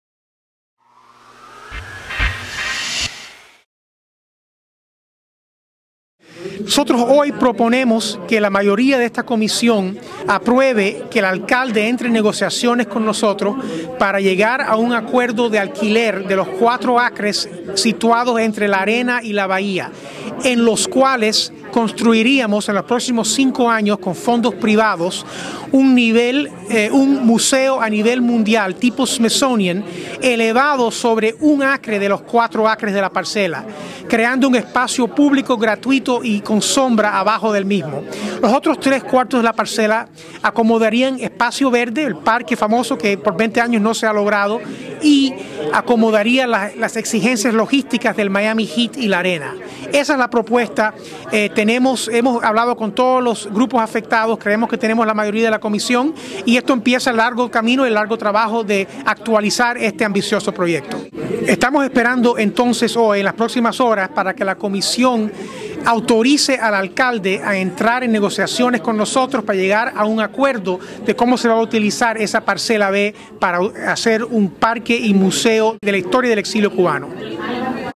Comisionados de Miami-Dade votarán sobre Museo del Exilio Cubano
Comisionados de Miami-Dade votarán sobre Museo del Exilio Cubano. Video por C.M. Guerrero / El Nuevo Herald